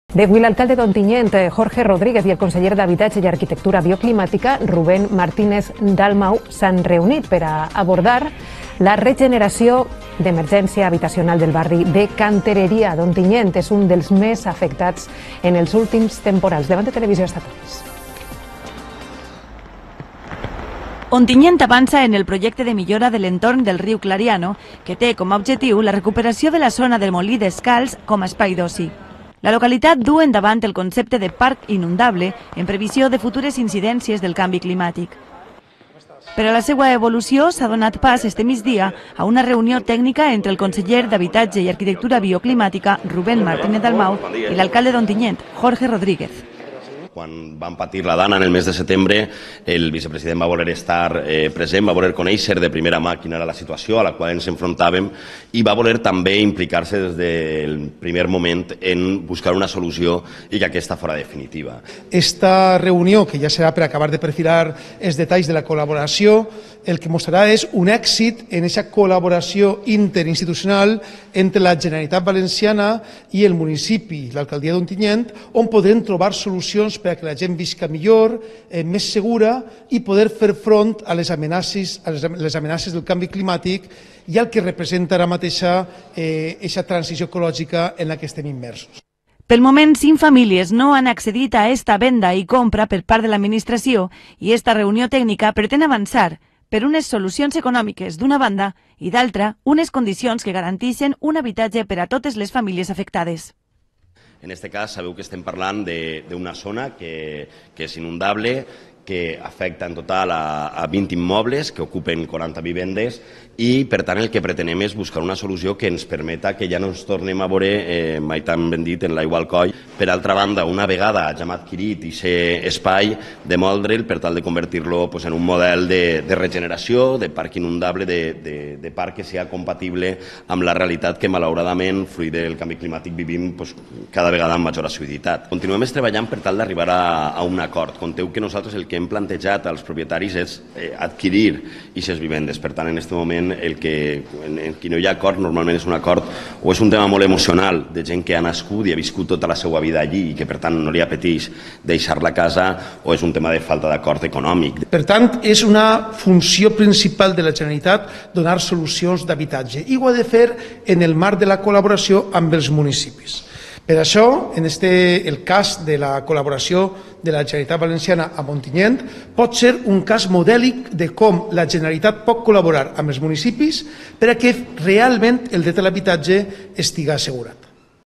Generalitat i Ajuntament estudien mesures d'urgència per a la renovació urbana de la Canterería.
Vídeo: Levante Televisión